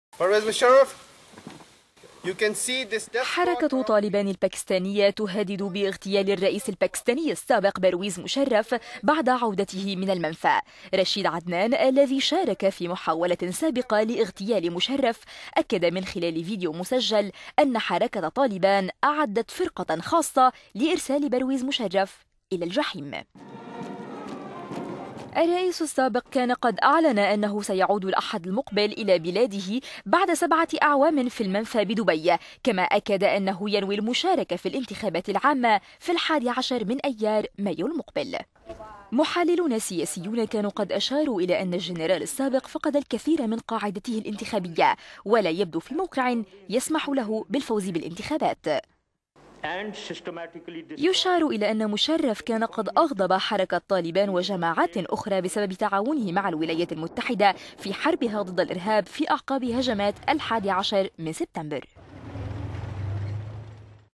حركة طالبان تتوعد بإرسال برويز مشرف الى الجحيم
حركة طالبان الباكستانية تهدد باغتيال الرئيس السابق برويز مشرف بعد عودته من المنفى ، رشيد عدنان الذي شارك في محاولة سابقة لاغتيال مشرف أكد من خلال فيديو مسجل أن حركة طالبان اعدت فرقة خاصة لإرسال برويز مشرف إلى الجحيم.
الرئيس السابق كان قد أعلن أنه سيعود الاحد المقبل إلى بلاده ، بعد سبعة أعوام في المنفى بدبي ، كما أكد  أنه  ينوي المشاركة في الانتخابات العامة في 11 ايار/مايو المقبل .
 محللون سياسيون أشاروا الى أن الجنرال السابق فقد الكثير من قاعدته الانتخابية ولا يبدو في موقع يسمح له بالفوز في الانتخابات .
كان مشرف  قد أغضب حركة طالبان وجماعات أخرى بسبب تعاونه مع الولايات المتحدة في حربها ضد الإرهاب في أعقاب هجمات 11 سبتمبر وبإطلاقه فيما بعد عملية قمع كبيرة على التشدد في باكستان تعرض على اثرها لعدة محاولات اغتيال نفذتها مجموعات اسلامية.